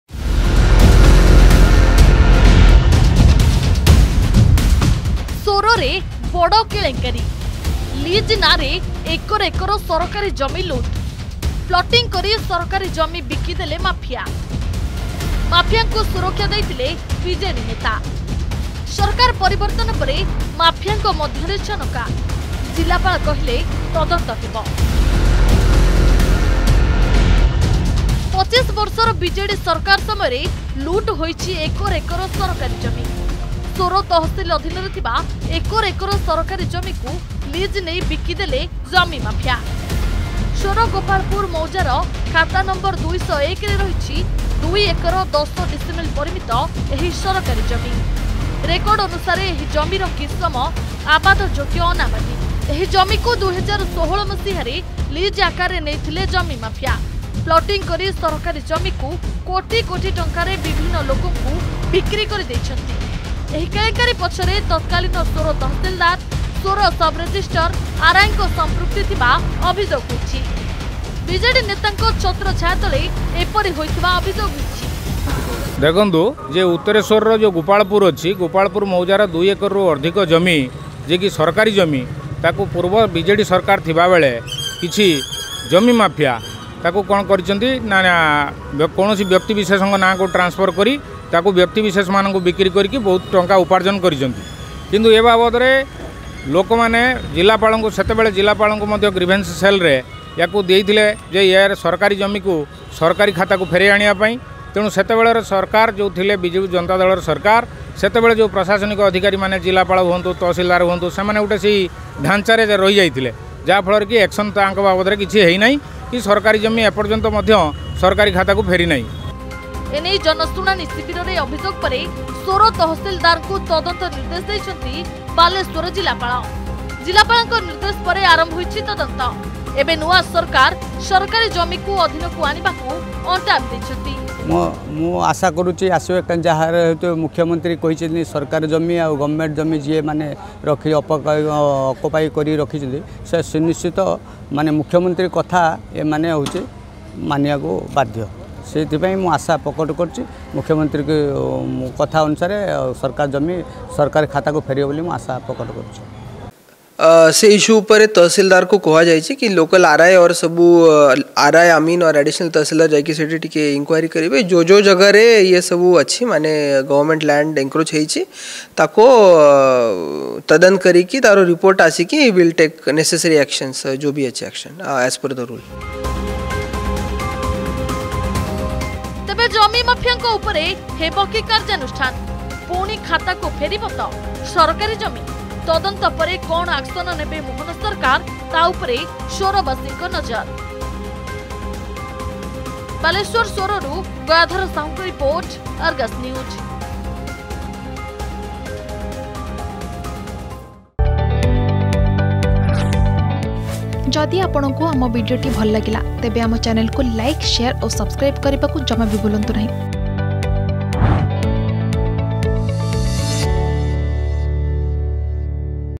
ସୋରରେ ବଡ କେଳେଙ୍କାରୀ || Balasore || Government Land Issue
ସୋରରେ ବଡ କେଳେଙ୍କାରୀ । ଲିଜ ନାଁରେ ଏକର ଏକର ସରକାରୀ ଜମି ଲୁଟ୍ । ପ୍ଲଟିଂ କରି ସରକାରୀ ଜମି ବିକି ଦେଲେ ମାଫିଆ । ମାଫିଆଙ୍କୁ ସୁରକ୍ଷା ଦେଇଥିଲେ ବିଜେଡି ନେତା । ସରକାର ପରିବର୍ତ୍ତନ ପରେ ମାଫିଆଙ୍କ ମଧ୍ୟରେ ଛନକା । ଜିଲ୍ଲାପାଳ କହିଲେ ତଦନ୍ତ ହେବ ।